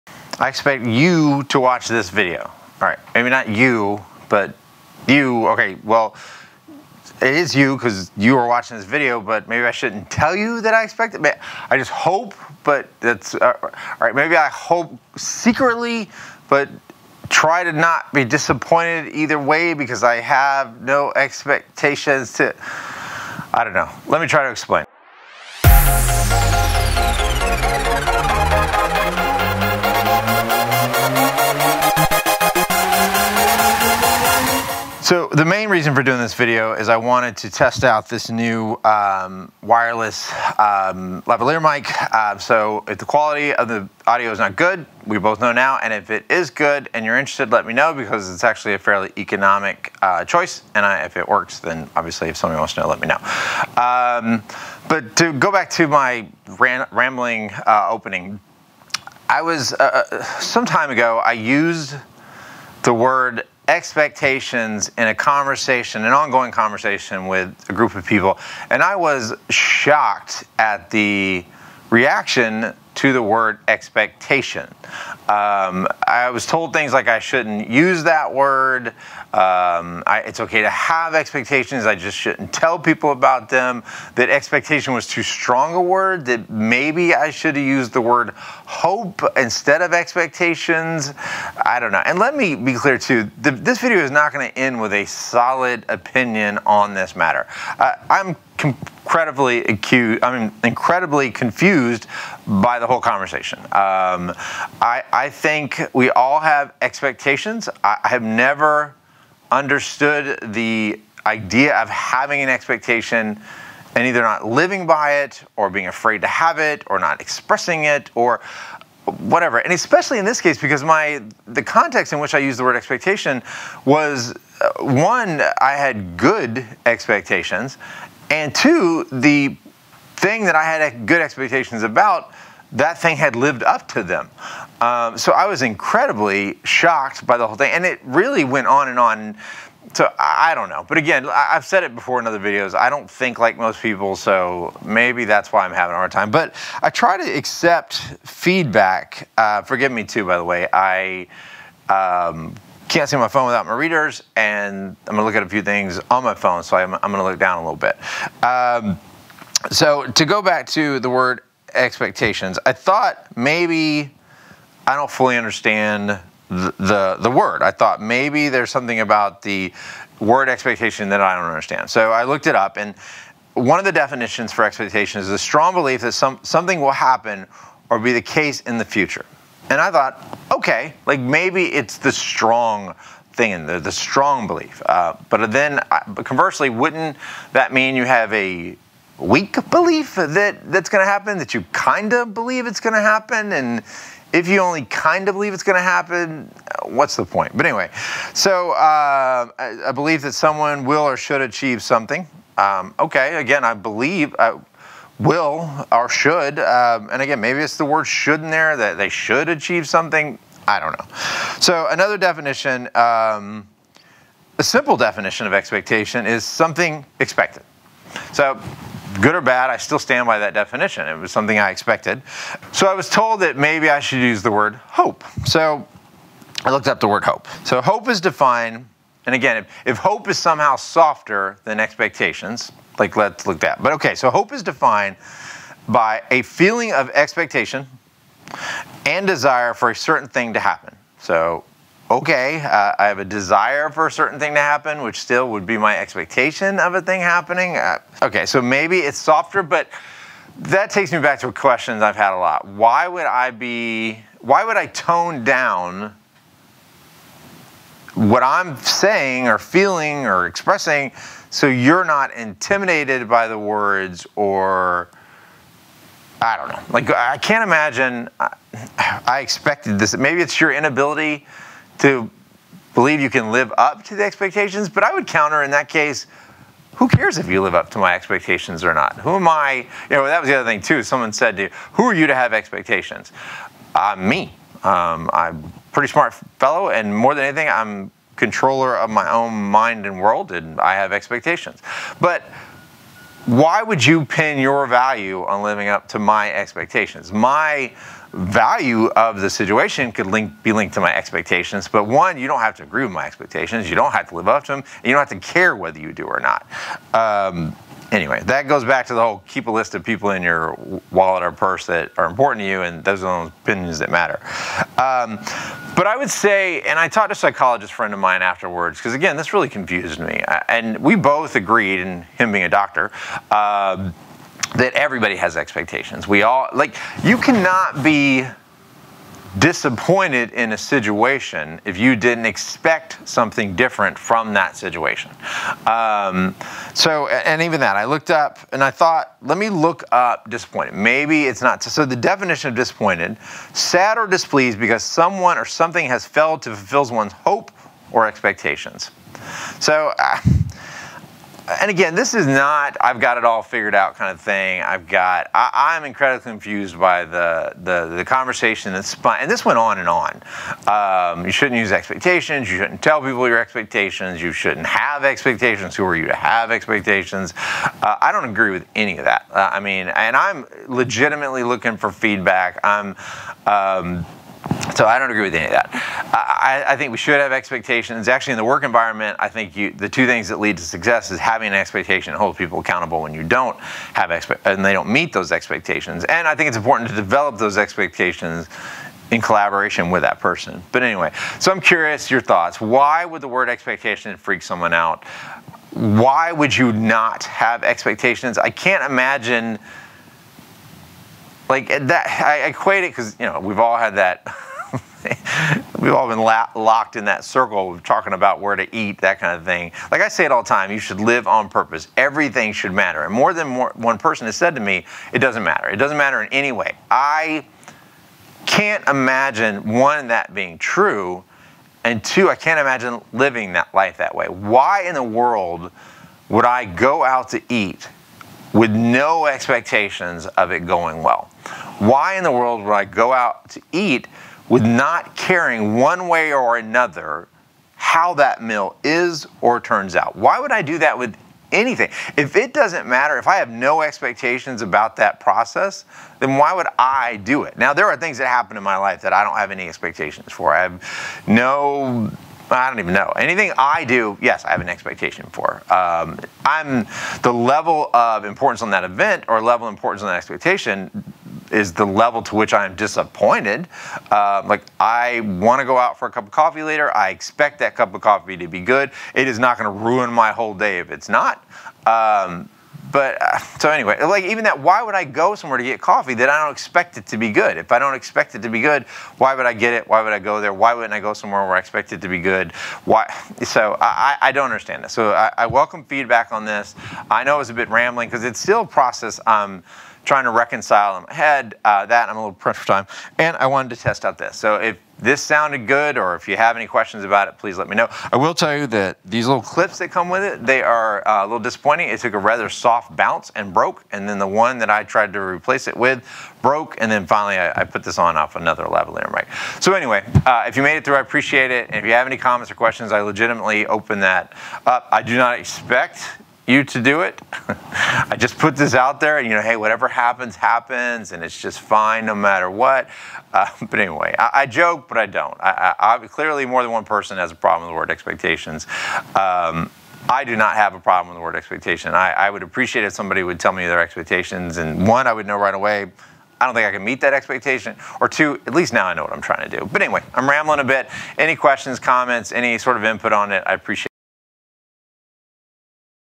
The problem with Expectations.
Do you have a problem with the word "expectations"? If so, why?  It is a bad thing to tell people your expectations or to have expectations?